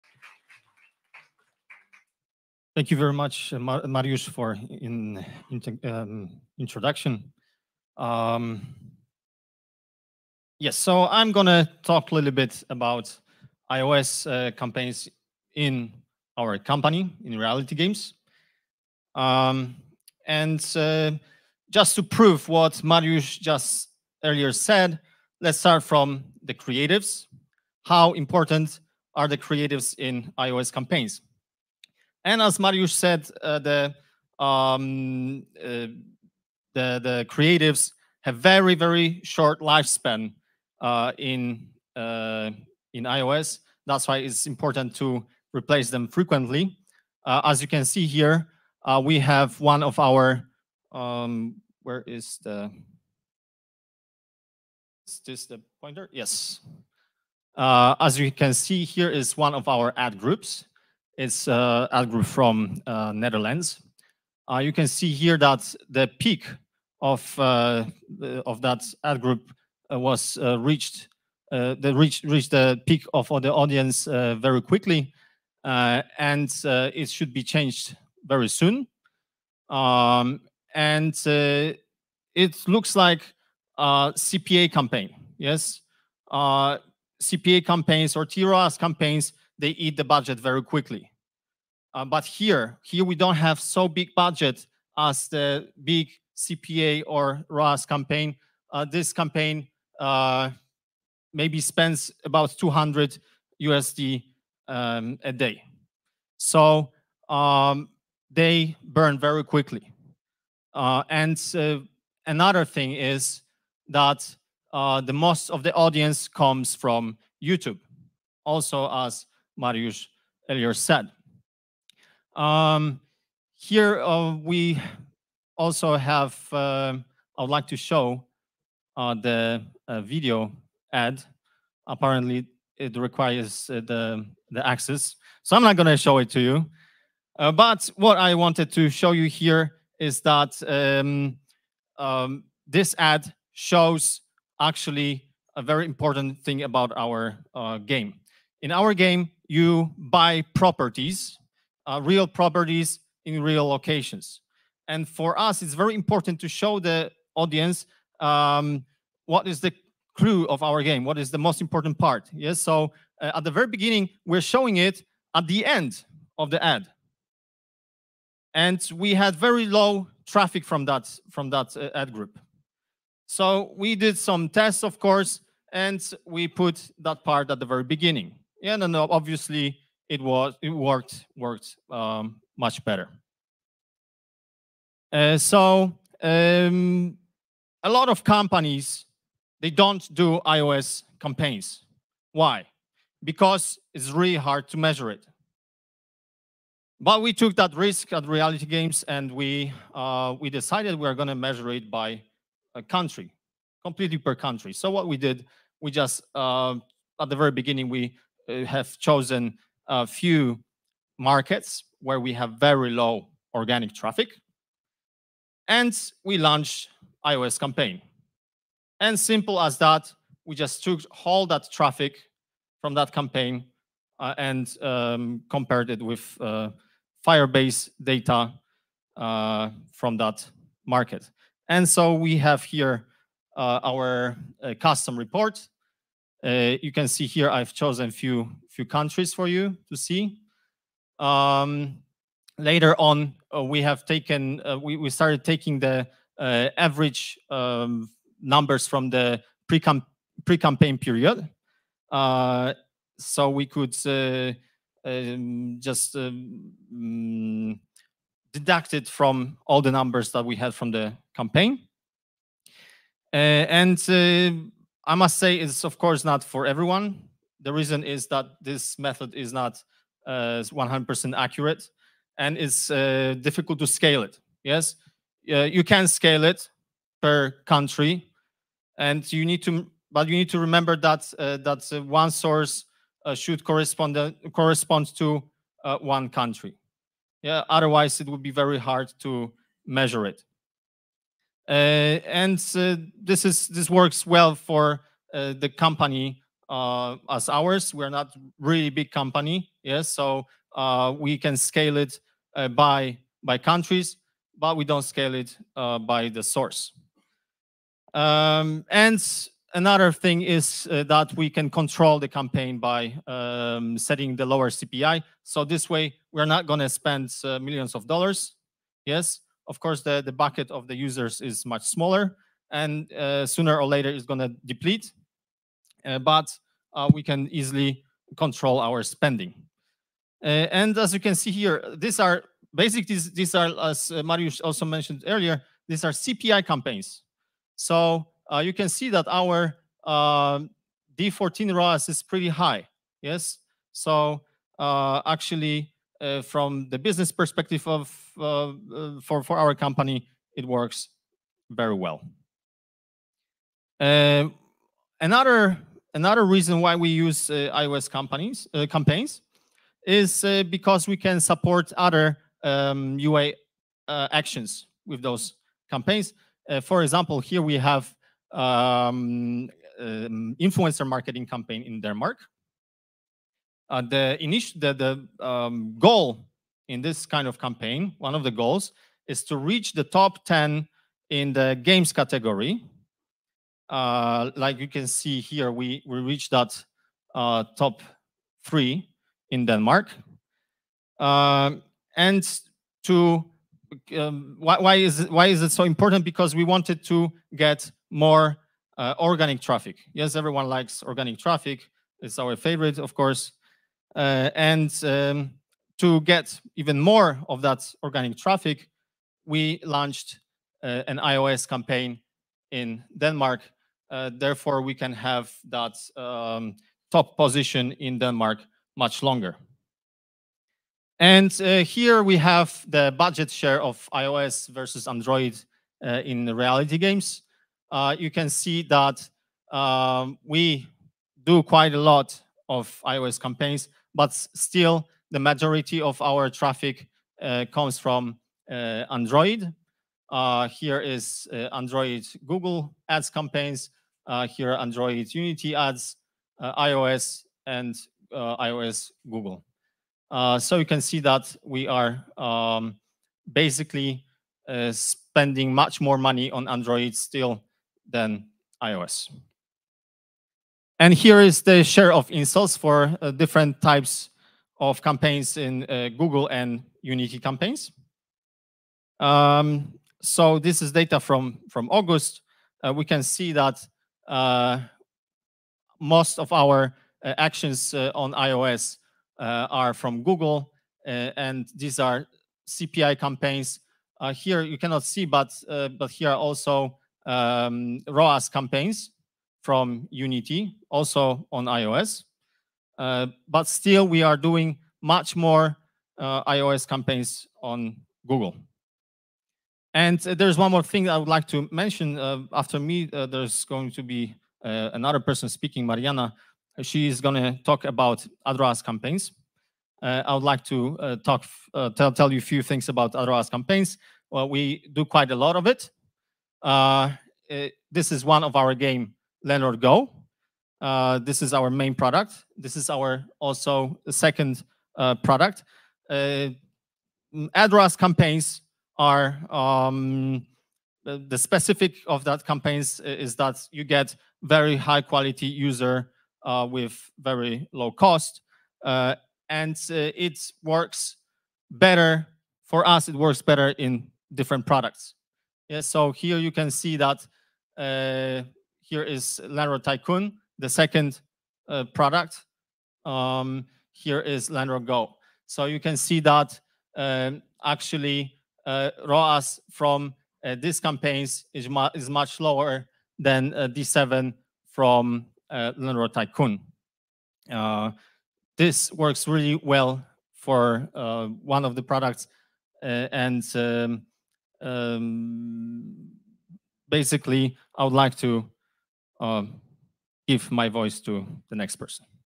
GameCamp: iOS in Google App campaigns. Reality Games case study
iOS in Google App campaigns. New Google App products (tROAS for ad revenue, App Campaigns for Engagement) in reality. When it works well?

Lukas Baranowski - Growth Manager, Reality Games

Different approach for iOS campaigns at Google - those that work and those do not work. Differences between approach towards Android vs iOS app campaigns at Google inventory. Real life experience from new Google app products (tROAS for ad revenue, App Campaigns for Engagement). Presentation based on examples and case studies.